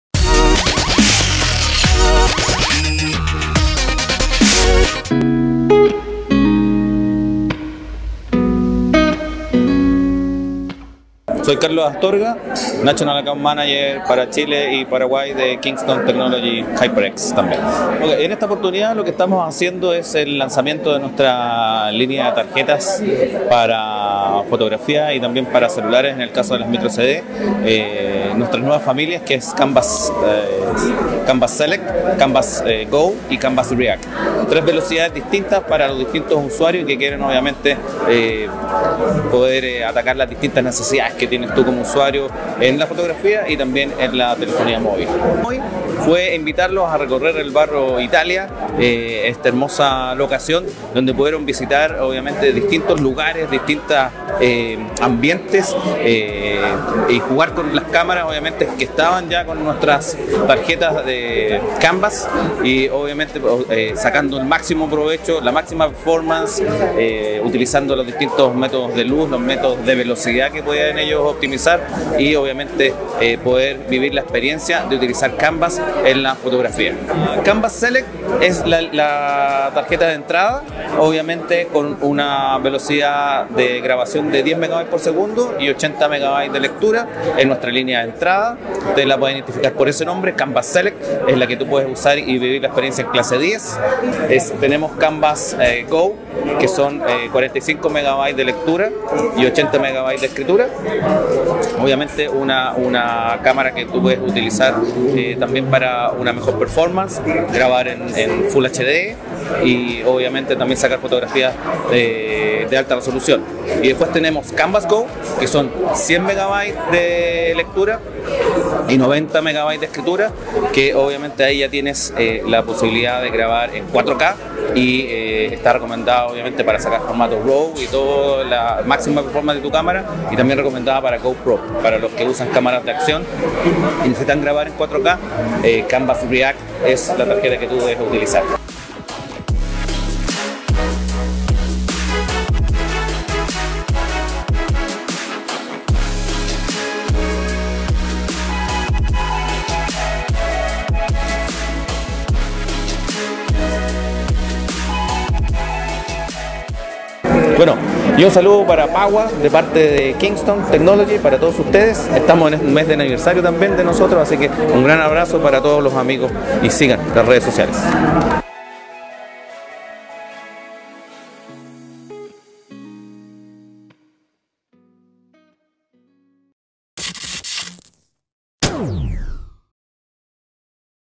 [COBERTURA] Kingston: Photo Tour Canvas 2018
PAWAdictos! Gracias a Kingston, estuvimos en el Photo Tour 2018, instancia que reunió a diversos fotógrafos para que probaran su nueva gama de memorias Canvas. Y aprovechamos la oportunidad para realizar una entrevista a Carlos Astorga, National Account Manager para Chile y Paraguay de Kingston y HyperX.

Nota: Estuvimos un tiempo sin videos. Nuestro equipo de edición dejó de funcionar, por lo que tuvimos que renovarlos. Estamos tratando de recuperar los videos, así que paciencia. Durante los siguientes días estaremos editando los videos pendientes y tratando de ponernos al día.

Video: @fuanichan
Edición: @snowpato